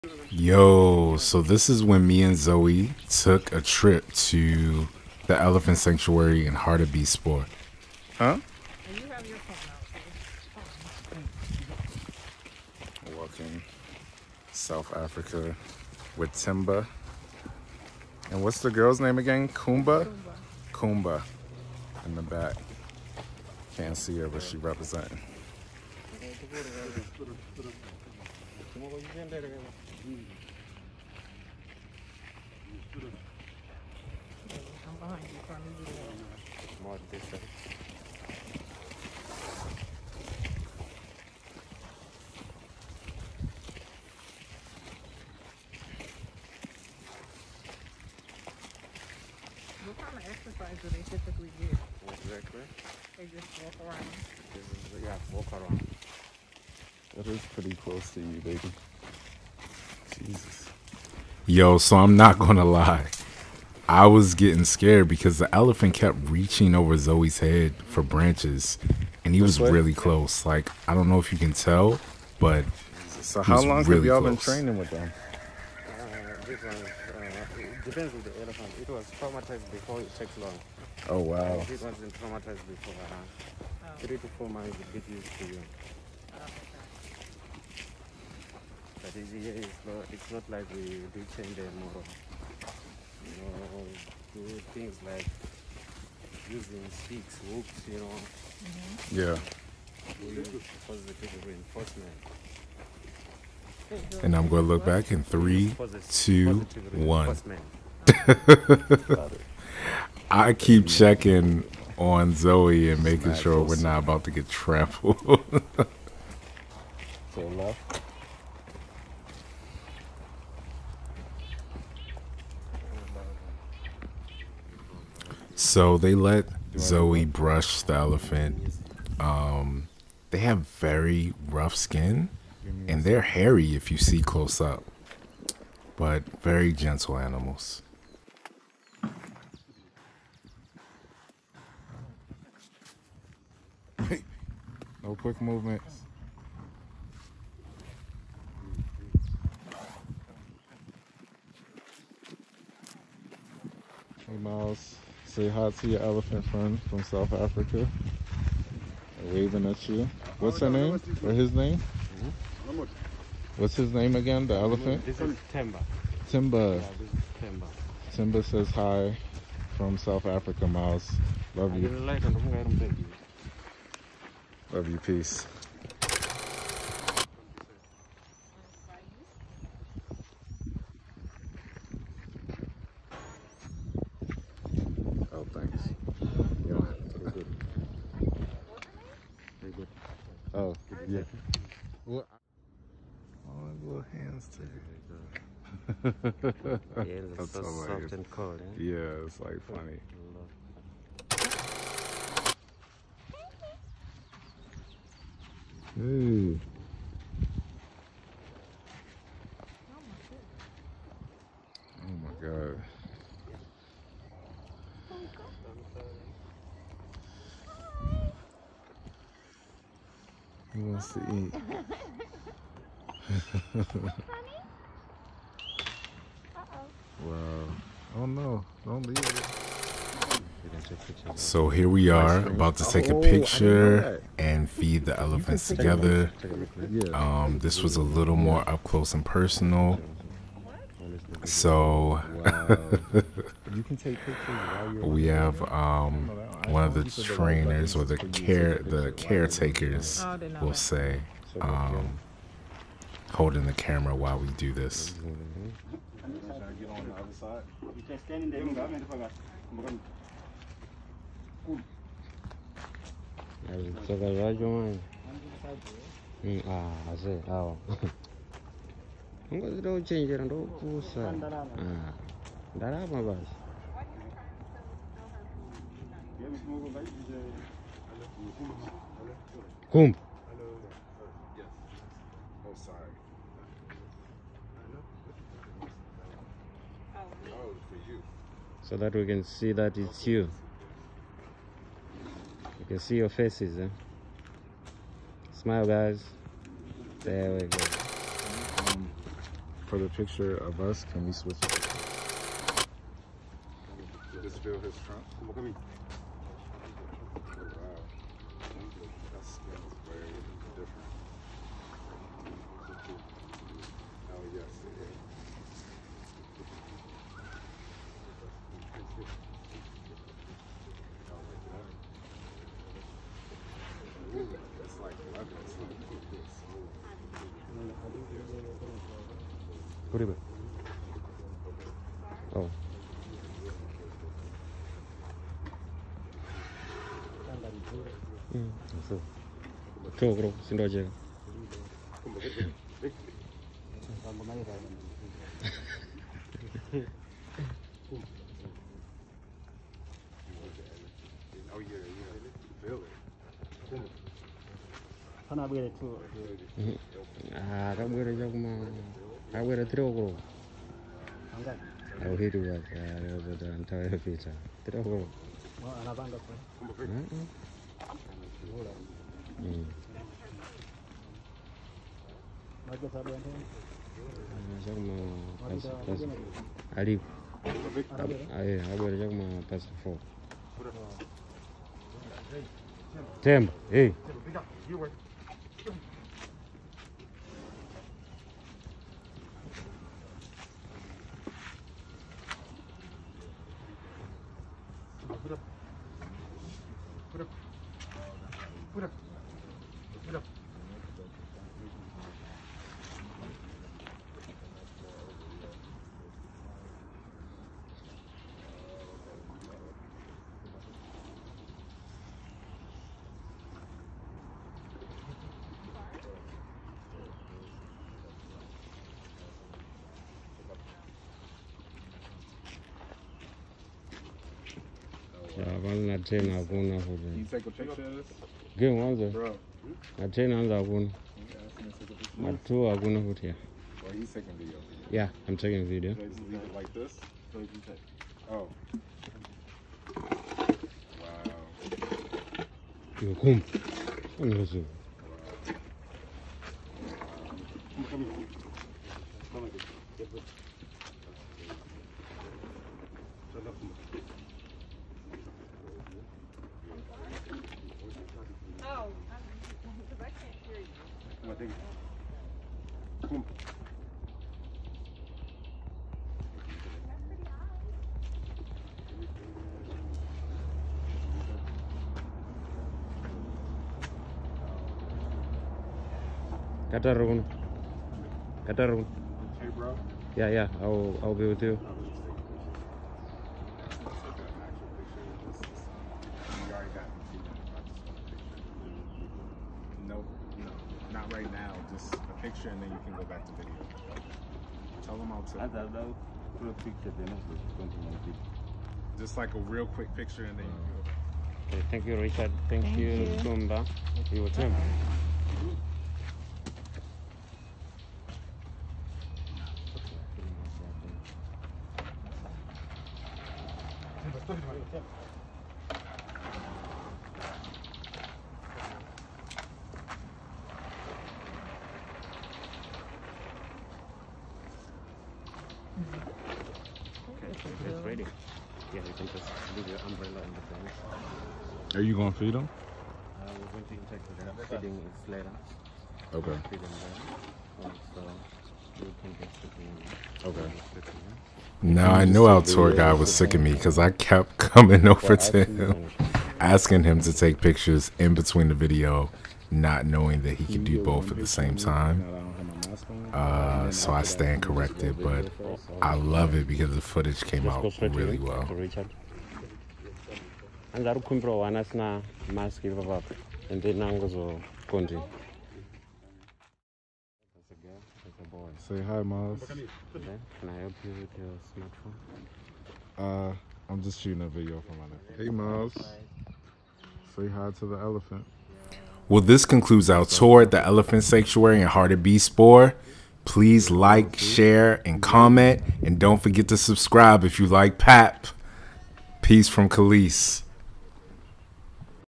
South Africa Elephant Safari: Wildlife Travel Vlog
Welcome to my travel vlog! Watch as we tour the African elephant safari in South Africa. We got to feed two teenage elephants Kumba is the female she was so sweet. They walked with us on a trail and were very close, I though we were going to get trampled lol. The African elphant is truly an amaxing gentle giant. I love South Africa and can't wait to return. You should definitely visit The South African Elephant Sanctuary in Hartbeeespoort.

blackvlogger big 5 wildlife